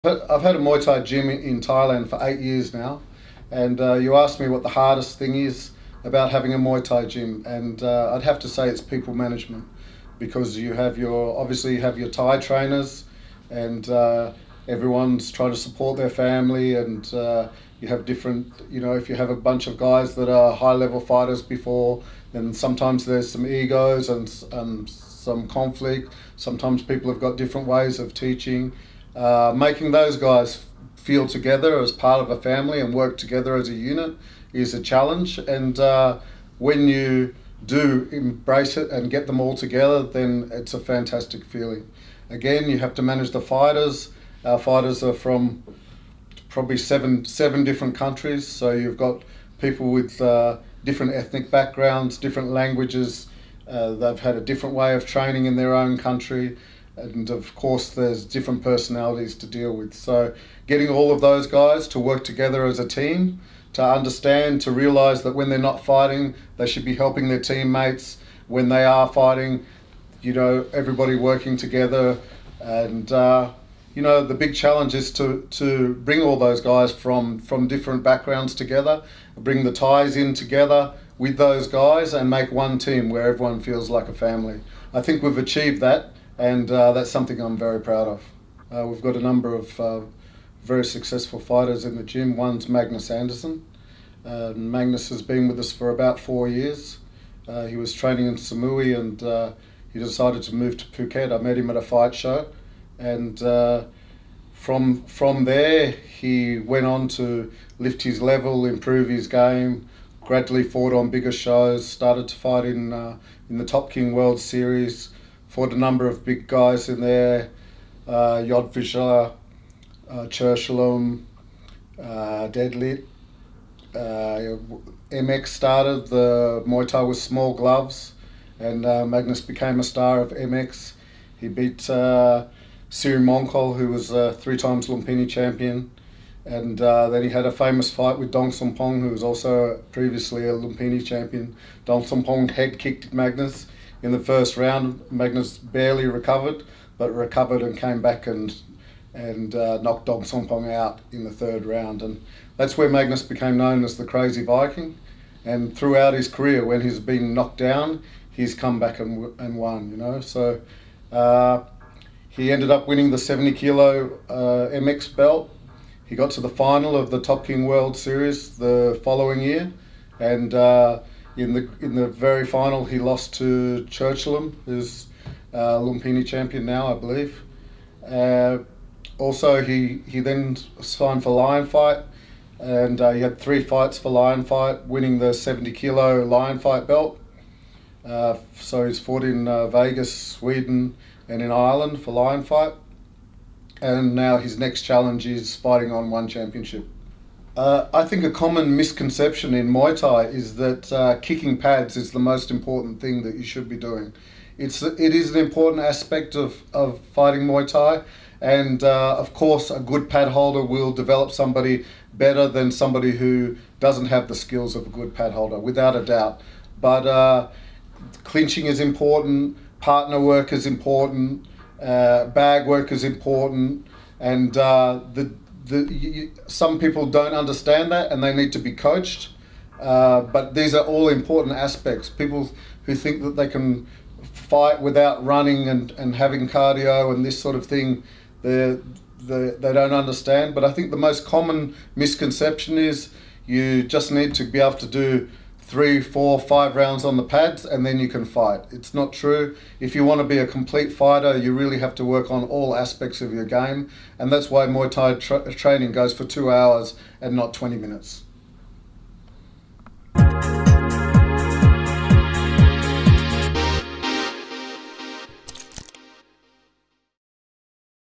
People Management With Tim Fisher
The real work behind the game.

Owner of Revolution Phuket, Tim Fisher, talks about the difficulties of managing a gym, the history of Magnus Anderson and what the real secret is to develop into a champion - hint it's more than kicking pads.

Perfect Your Trade